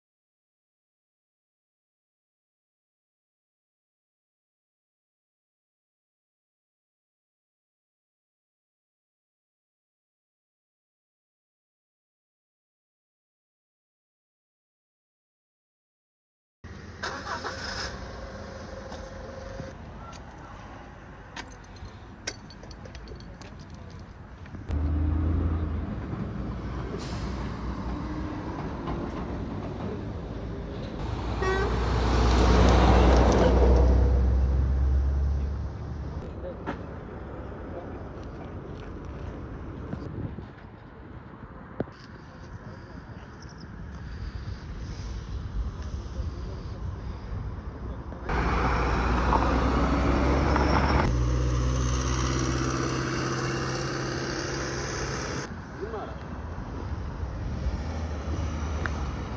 Hafif ticari araç ile otomobilin çarpıştığı kaza anı güvenlik kamerasına yansıdı
Zonguldak-Ankara karayolu Gökçebey yol ayrımında trafik kazası meydana geldi. Hafif ticari araç ile otomobilin çarpıştığı trafik kazası, güvenlik kamerasına yansıdı.